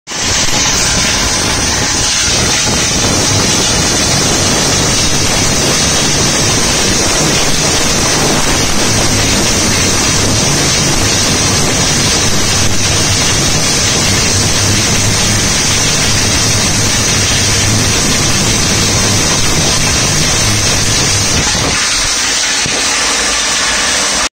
WindHexe Build prototype V02 - Cans in Tornado in a Can - 13 July 2023
Tony Jabony's realtime video of Al cans in the WindHexe Build prototype V02